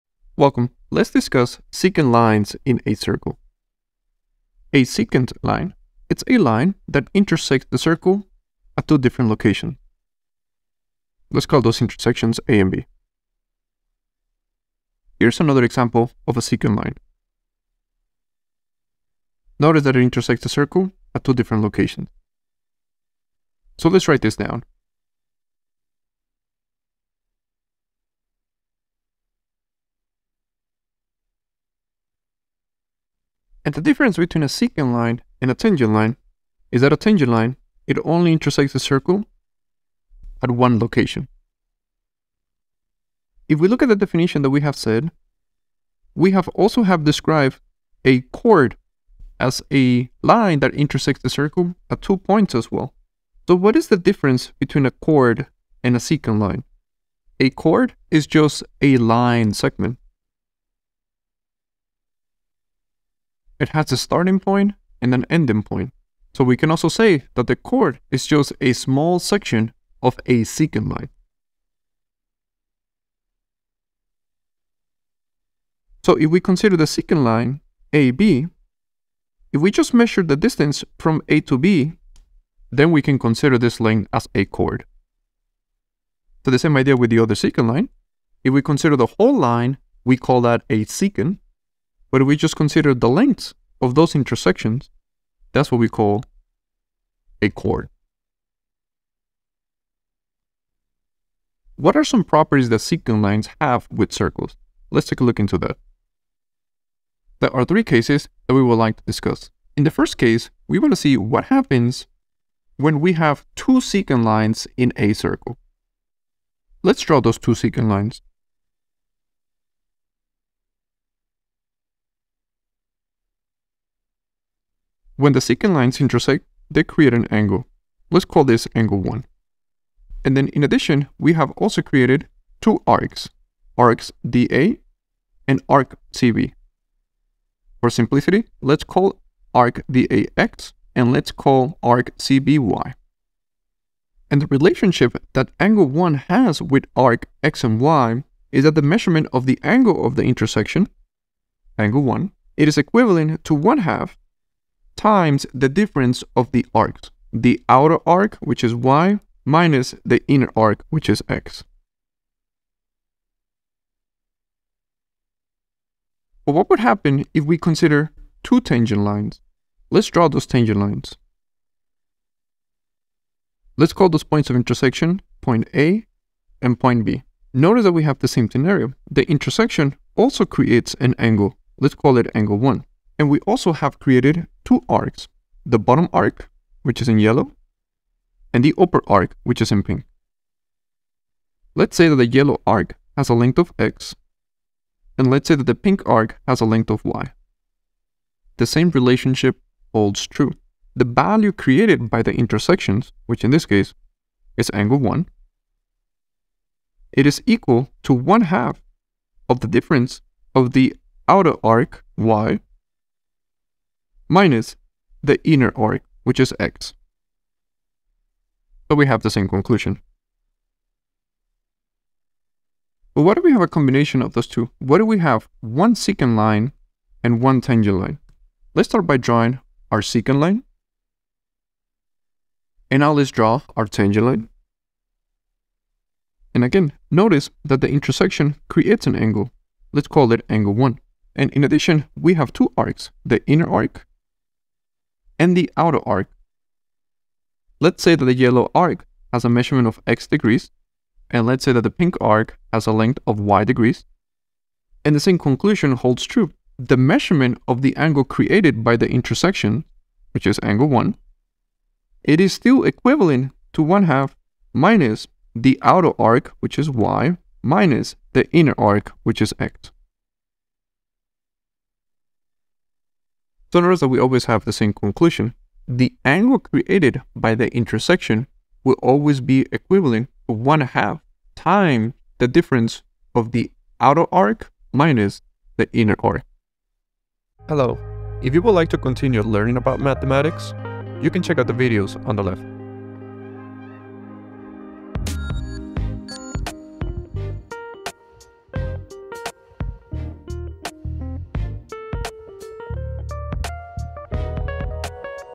angles outside of circles - Properties and explanations (Mastering Geometry)
Angles outside of circles is a lesson that will show you how to identify and understand the properties that occur when we have angles outside of circles. These outside angles are created when tangent and secant lines intersect, but what measurement does these angles have? At the end of the lesson you should be able to answer this question given the properties of angles outside of circles.

⏰Timestamps⏰
 0:00 Introduction
0:04 What is a Secant Line?
0:52 Difference between secant and chord
1:50 Angle created by two intersecting secant lines
2:45 Angle created by two intersecting tangent lines
3:41 Angle created by intersecting secant and tangent lines